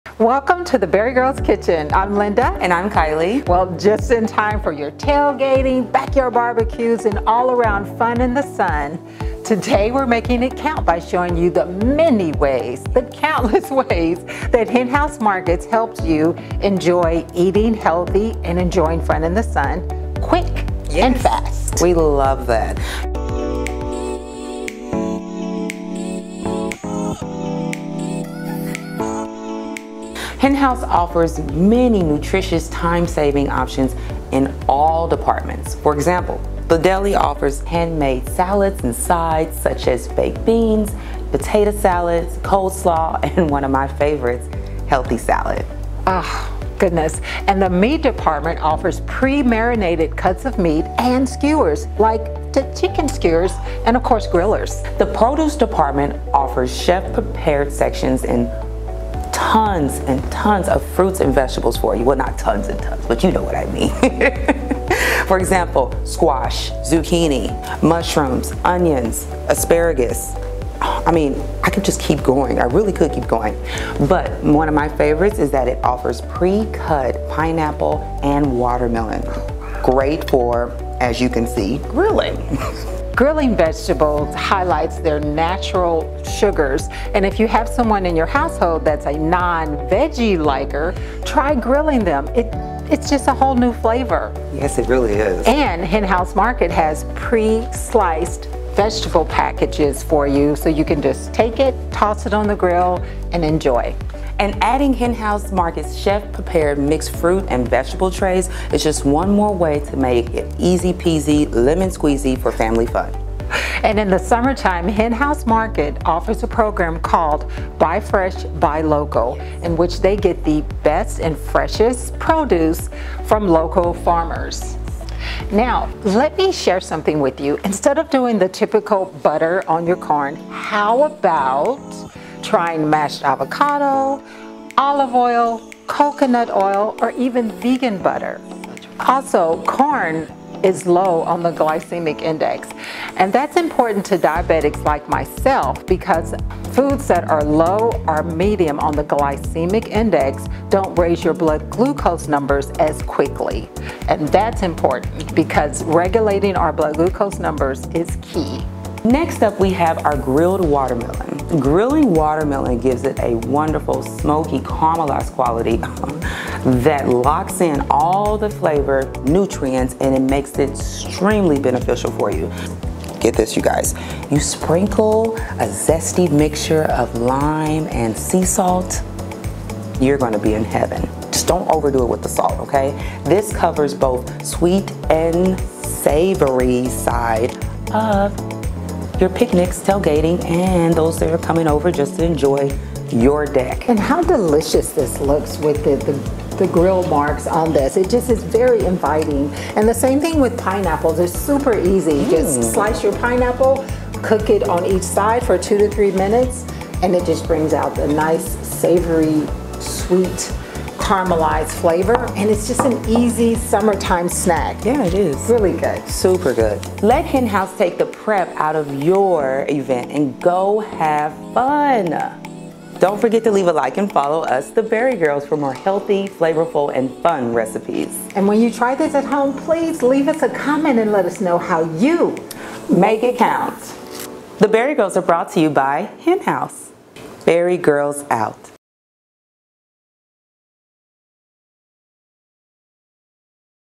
The Bari Girls: Easy Grilling Sides
These Hen House sides are picnic perfect! 🧺 Enjoy the fresh tastes of summer with inspiration from the Bari Girls!